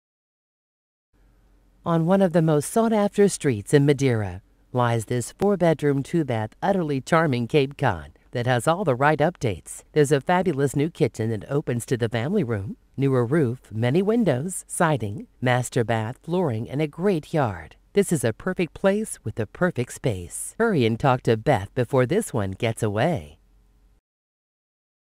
Home for sale in Madeira, OH | $387,500
One of the most sought after streets in Madeira. This utterly charming Cape Cod has all the right updates! Fabulous new kitchen opens to fam rm. Newer roof, many windows, siding, mstr bth, flooring. Great yard. Perfect place, perfect space. Hurry